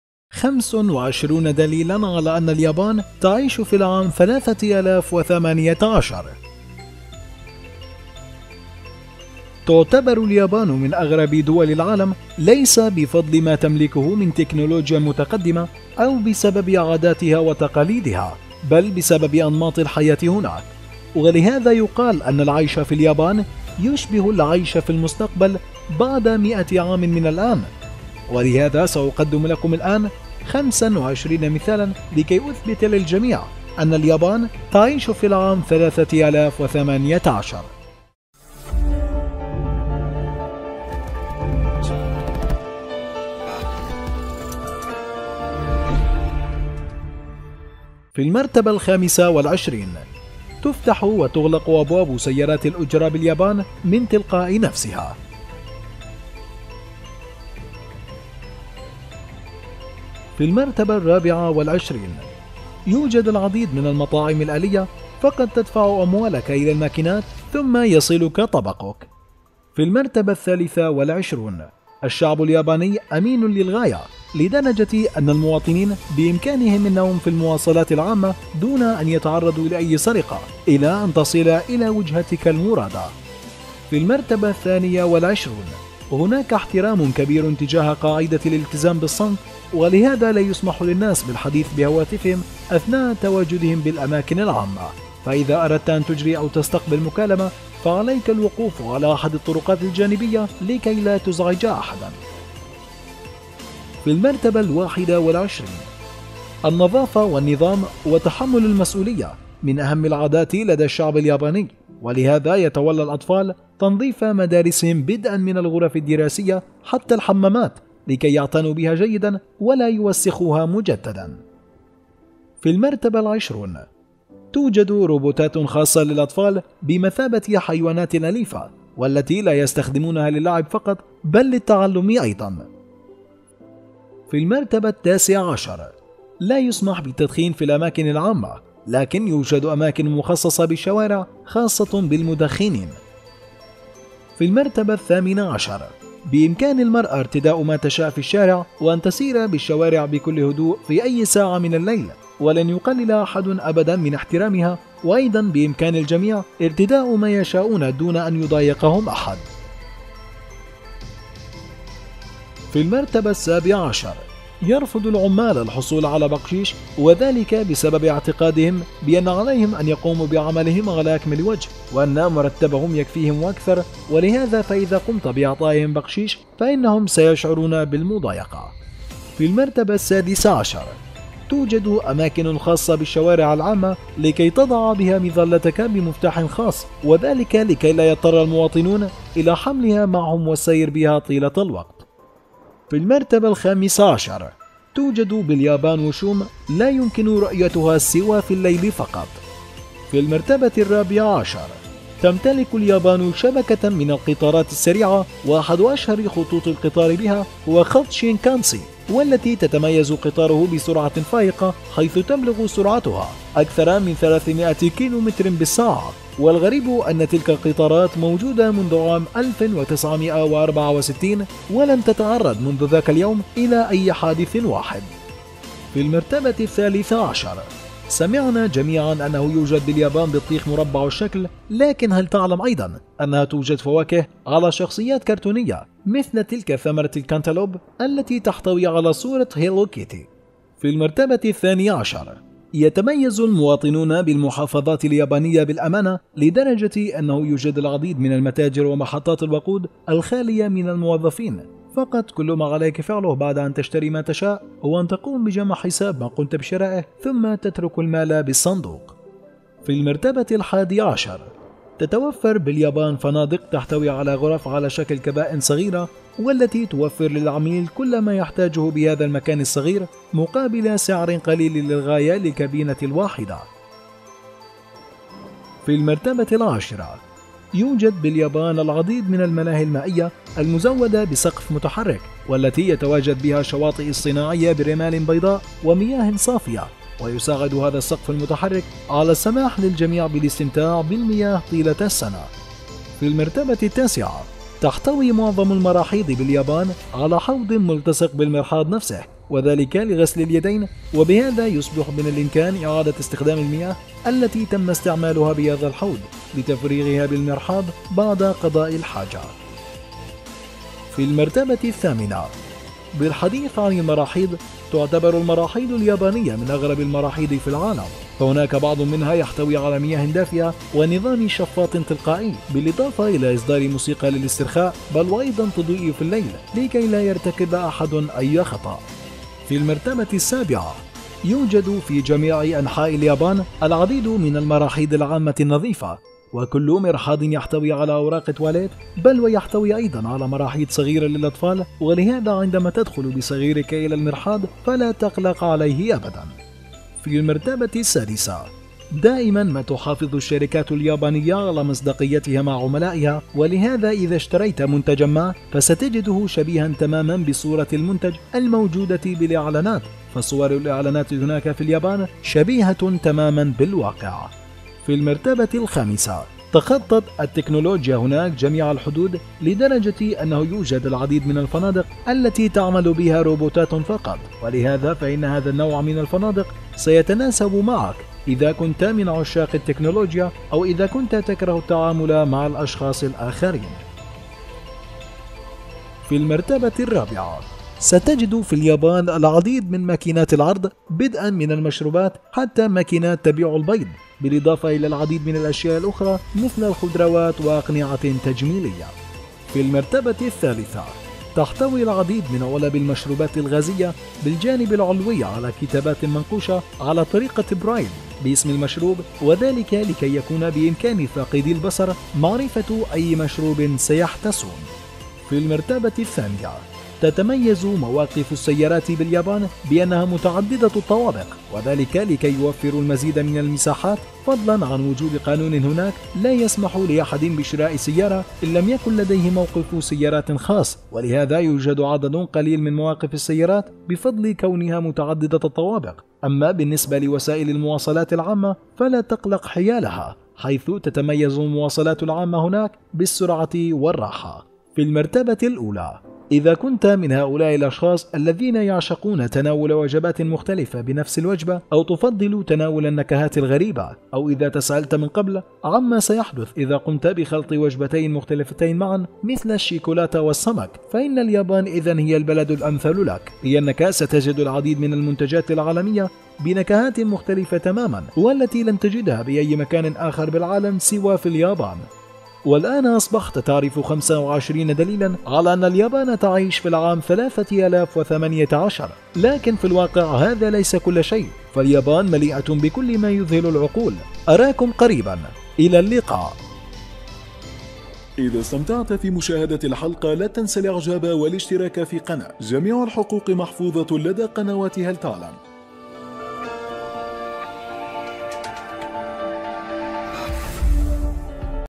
25 دليلا علي ان اليابان تعيش في العام 3018 !!!
مرحبا بكم جميعا ، اتمني ان تكونوا بأفضل حال،  تعتبر اليابان من اغرب دول العالم،  ليس بفضل ما تمتلكه من تكنلوجيا متقدمة او بسبب عاداتها و تقاليدها، بل بسبب انماط الحياة هناك،   ولهذا يقال ان العيش في اليابان يشبه العيش في المستقبل بعد مئة عام من الان ، ولهذا ، سأقدم لكم الان 25 مثالا لكي اثبت للجميع ان اليابان تعيش في العام 3018...